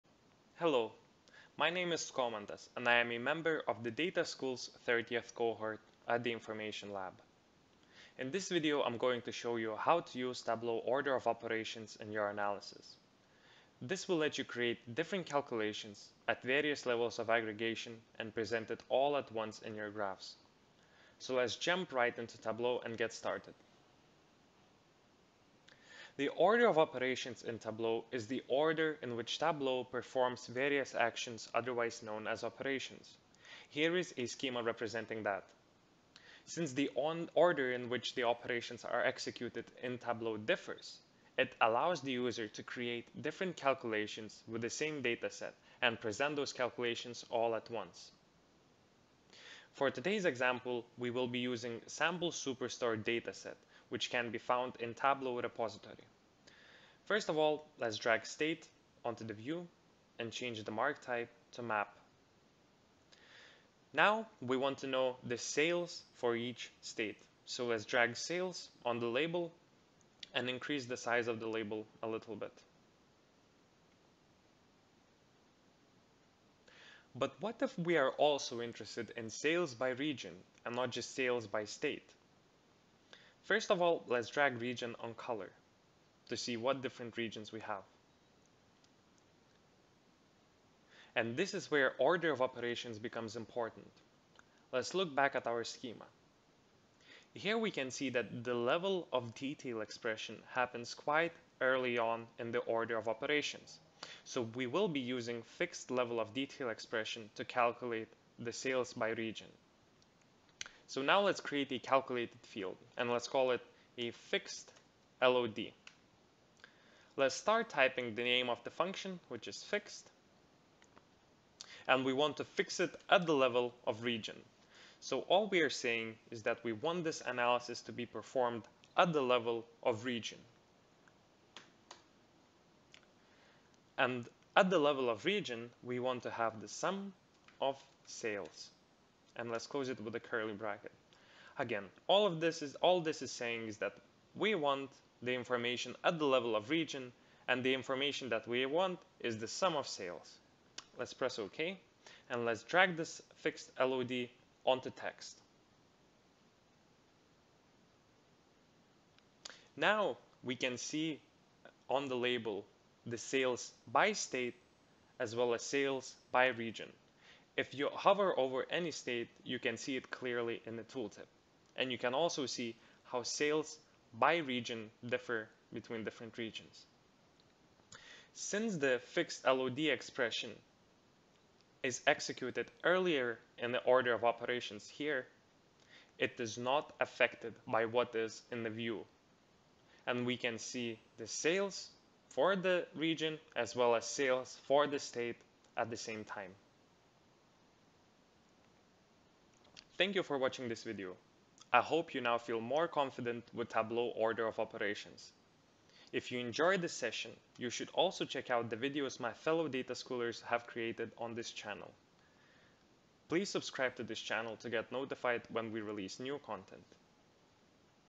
How to in Tableau in 5 mins: Order of Operations
Learn how the order of operations impacts how calculations are executed in Tableau. In this video get to grips with Tableau's order of operations so you're never surprised with unexpected results to your calculations with Skomantas Tamulaitis.